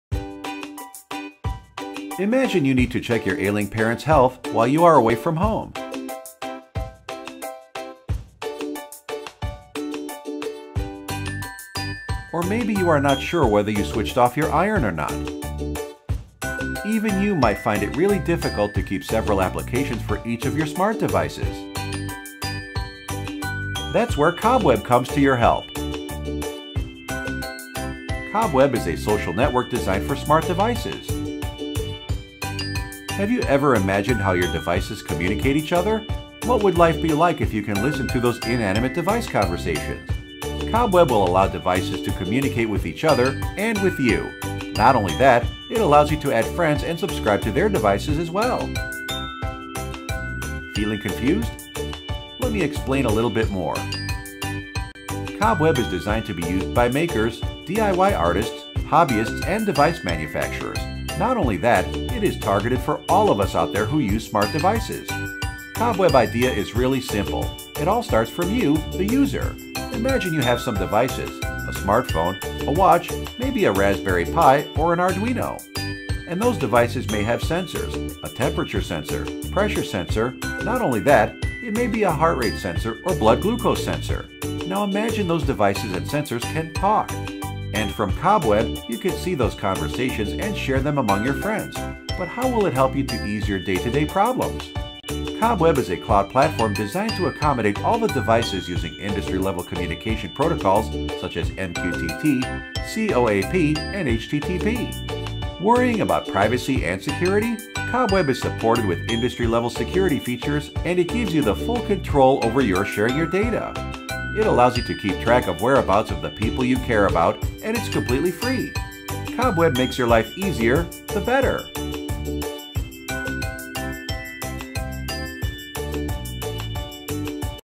Cobweb IO - A Social Network for Internet of Things
In the near future, the smartest products will connect not just to the Web but also to a whole network of
people.There is a new wave of connected objects on the horizon. The real change will happen when products
of Things. To take advantage of this shift, you need to start thinking about the social life of your products.
some vector graphics are designed by Freepik.com
some icons are made by Freepik from flaticon.com